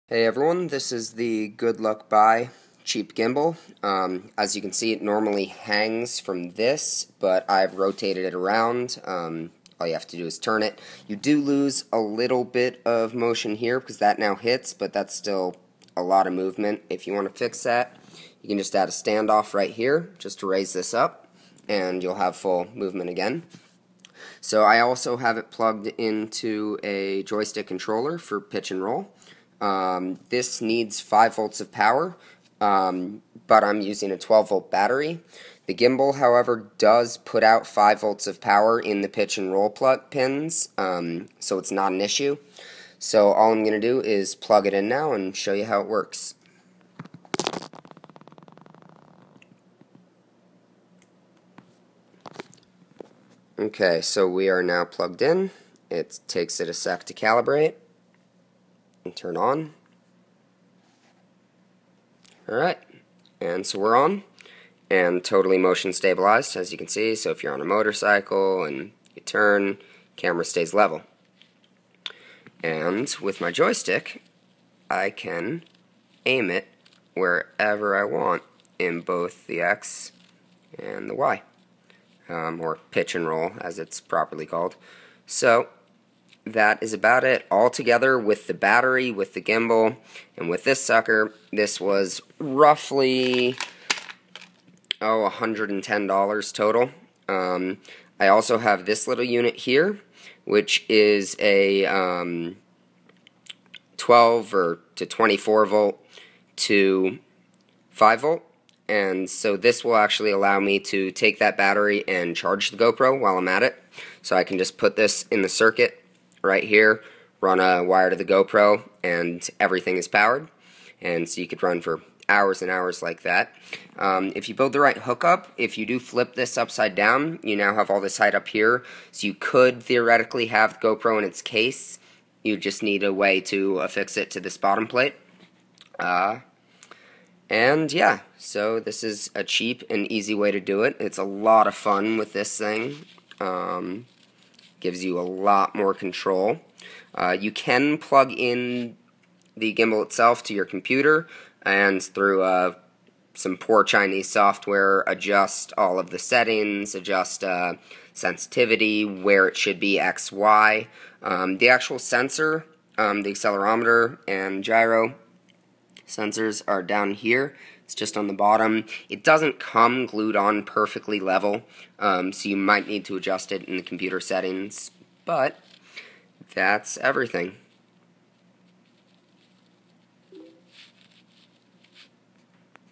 Cheap goodluckbuy gimbal for land use with joystick pitch roll control
This is the $60-$70 (depending on the day) "DJI Phantom Brushless Gimbal" from goodluckbuy. I will be using this for land use (hiking, motorcycles, etc) so I wanted to see how it would work with handheld control for pitch/roll as well as bottom mounting.

Current total for all components is $97

Battery (this is what I ordered though not the exact one delivered, specs the same though) edit: the battery died really quickly, you get what you pay for. You're better off buying a respected battery!

To clarify my comment in the video about the case, nevermind, wouldn't work it's really too big. It would be possible I suppose if you go into the gimbal's settings with a computer and adjust it for the new weight but it's really not designed for that.